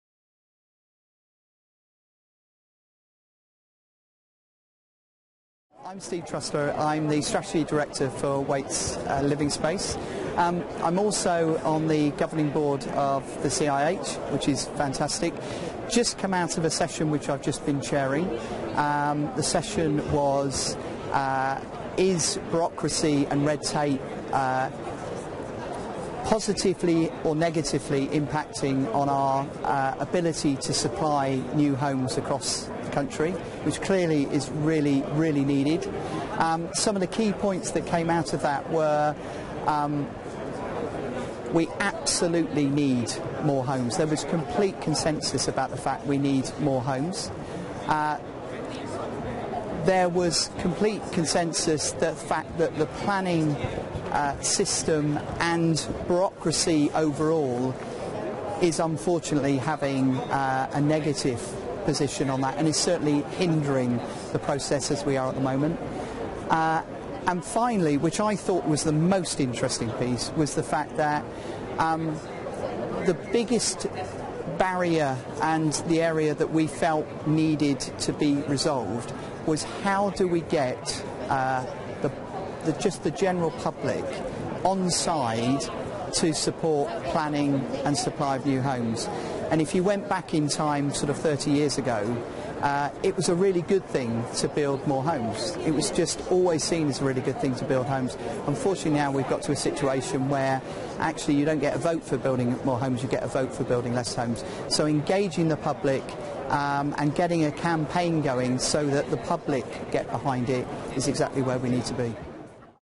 Steve Trusler - Cutting through 'red tape' and bureaucracy
Steve Trusler, fresh from chairing a session, looks at some of the key points to emerge during discussions